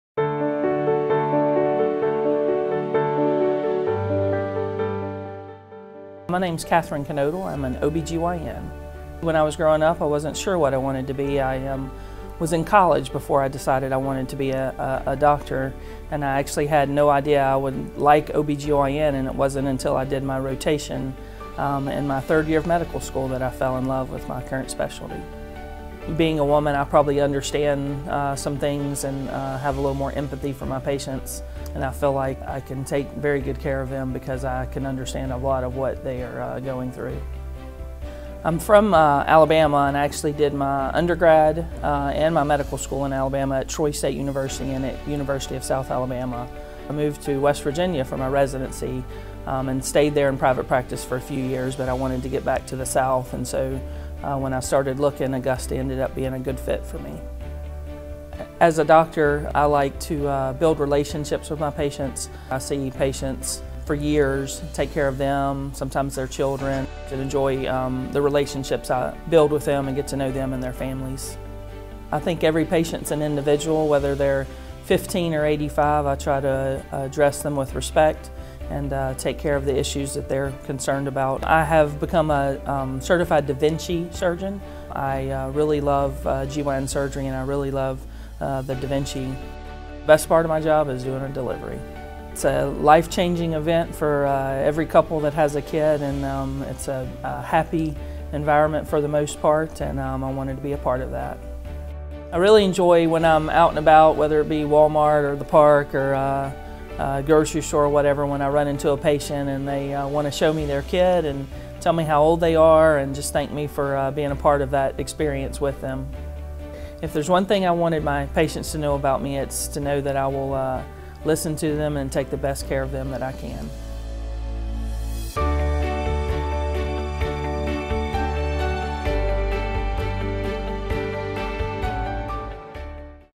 Dr. Kathryn Knodel
Dr. Kathryn Knodel is a physician with Augusta OB/GYN Specialists.